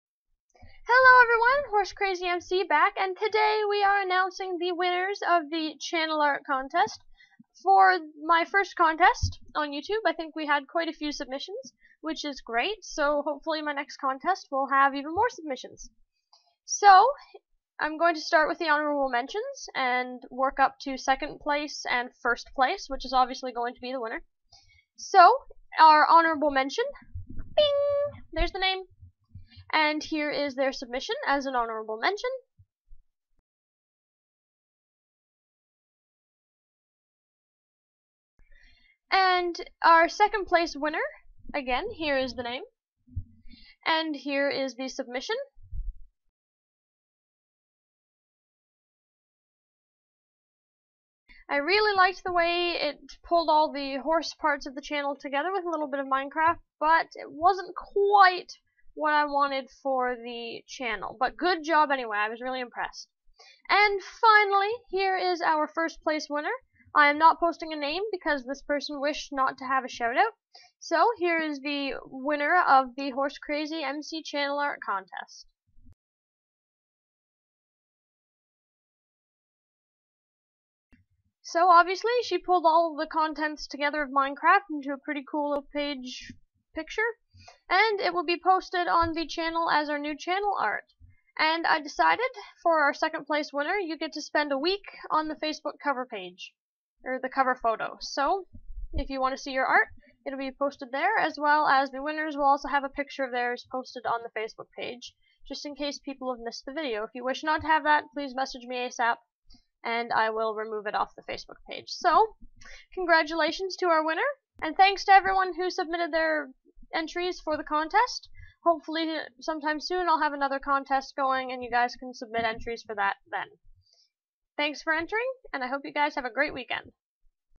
HorsecrazyMC Channel Art Contest Results
Here it is, the results of my first youtube contest - HorsecrazyMC Channel Art.